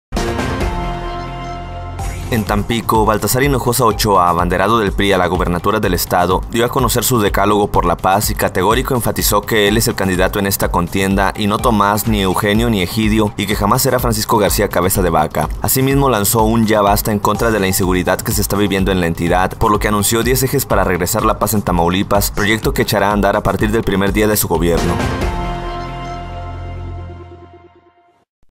Se desmarca Baltazar de ex gobernadores
El abanderado del PRI a la gubernatura del estado dio a conocer su decálogo por la paz y categórico enfatizó que él es el candidato en esta contienda y nadie más.